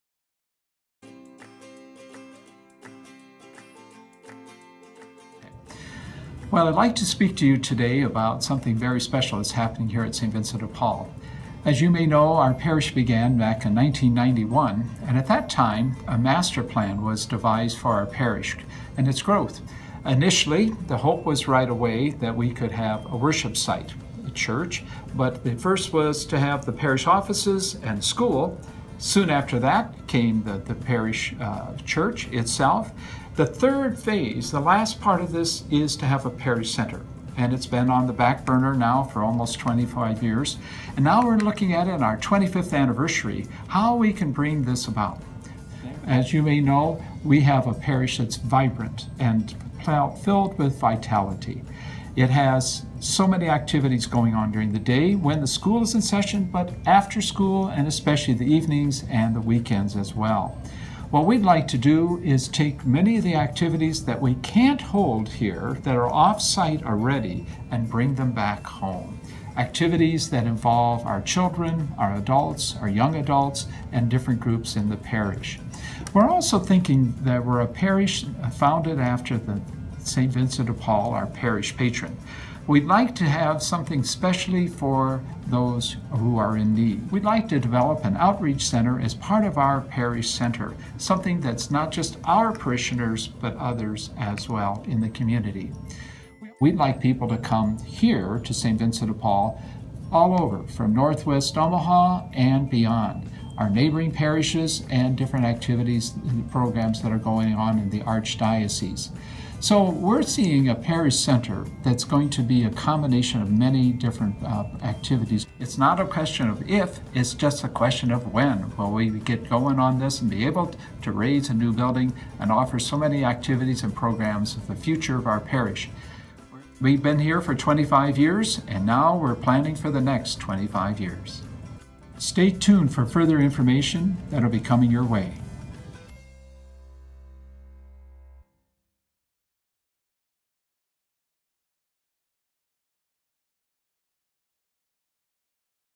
SVdP Parish Center Introduction
Fr. Dan introduces the plans for the future parish center at St. Vincent de Paul Parish, Omaha.

We are excited to share the progress to complete our 1992 Master Plan to build a Parish Center on the west side of our Church.
Join us for one of the following preview sessions to view the architectural renderings and get a glimpse into the new SVdP Outreach Center that will serve Northwest Omaha.
Come learn about volunteer opportunities to assist with the campaign.
Monday, Oct. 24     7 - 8 p.m. Family Room
Tuesday, Oct. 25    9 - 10 a.m. St. Vincent Room
Thursday, Nov. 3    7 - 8 p.m. Media Center

Please RSVP to Parish Office